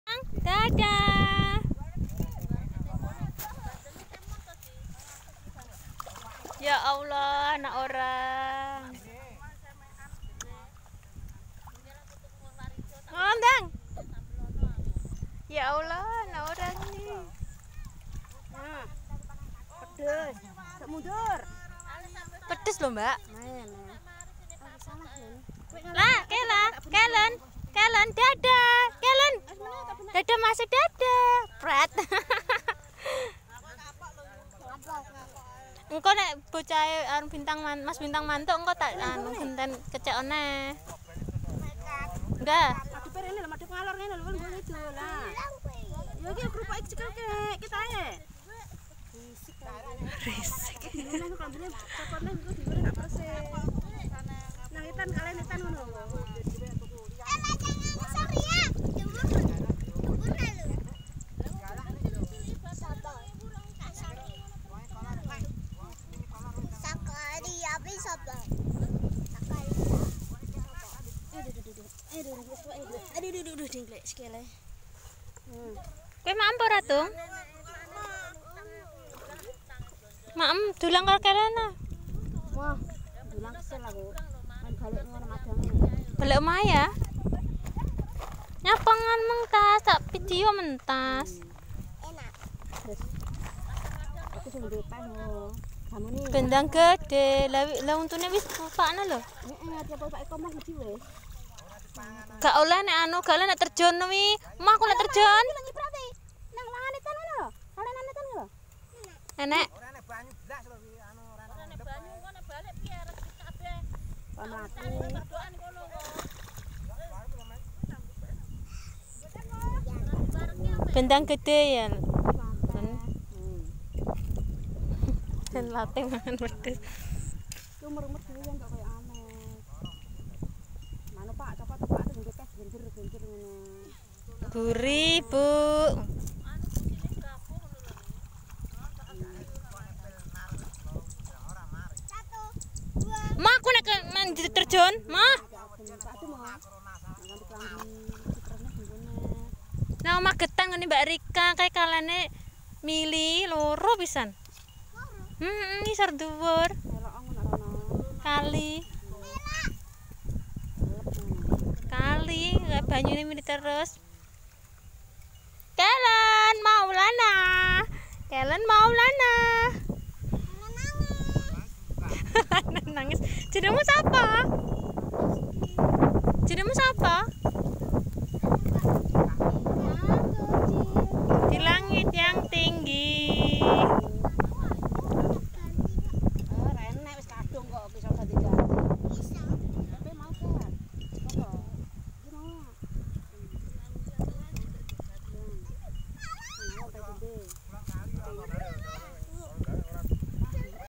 Balita bermain lumpur disawah#TKIKoreaTV
Kesempatan renang di sawah dan bermain lumpur  tidak dilewatkan oleh seorang balita di dusun Ponjen kab Ngawi, manakala diajak bibinya mencabut kacang tanah disawah